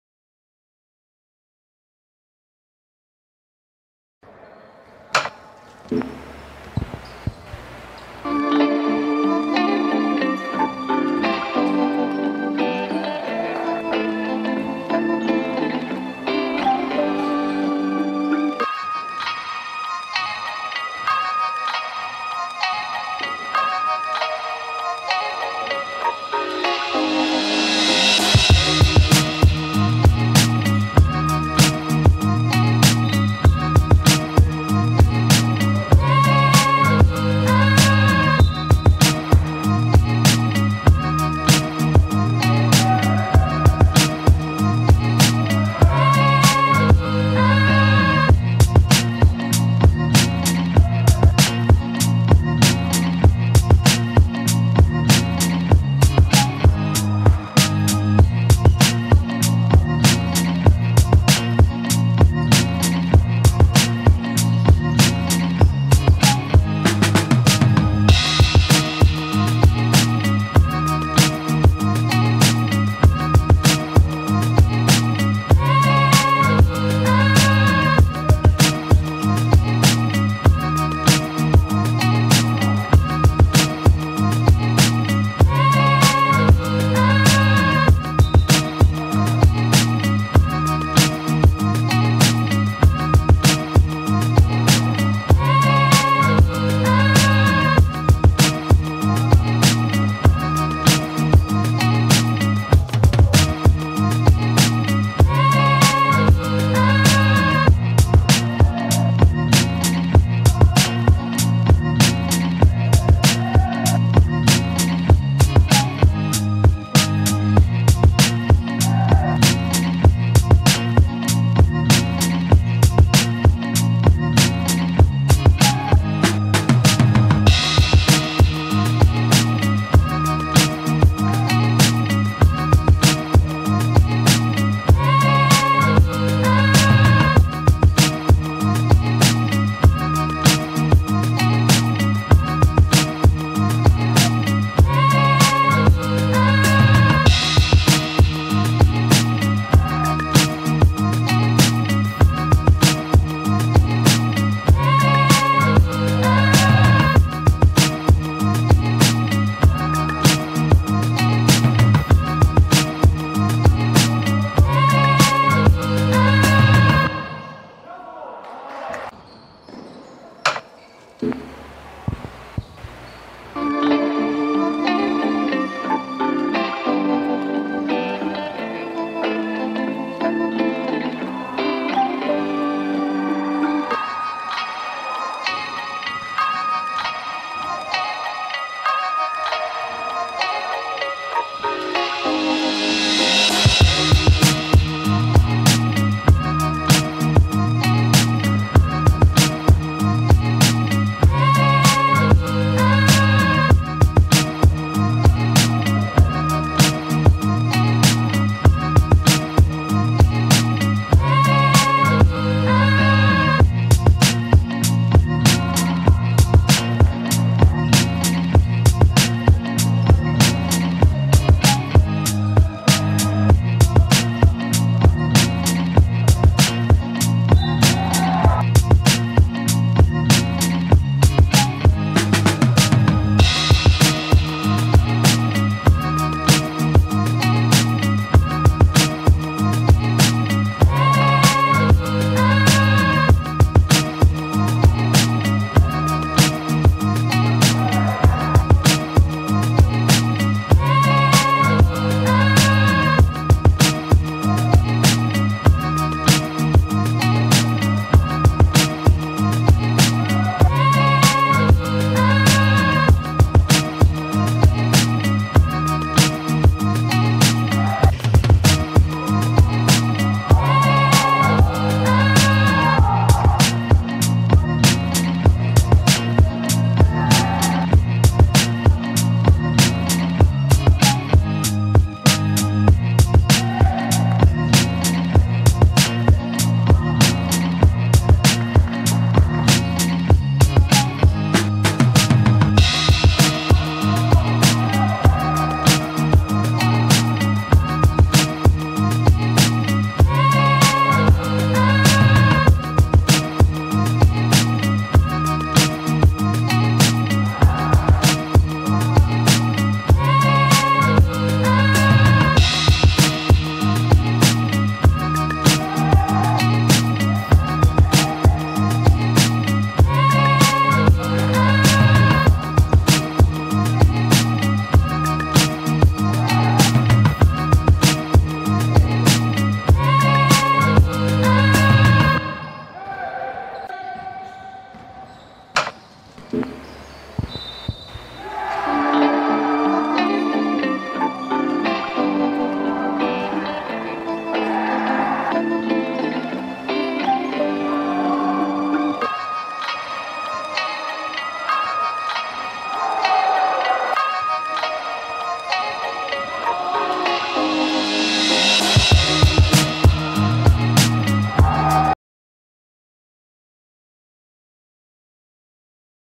04 11 Baylor vs North Carolina – BIG 4 Jesień 2023 r
Baylor 80:77 North Carolina
R.Kurylo 17pkt 19zb
M.Piątkowski 15pkt 5zb 5ast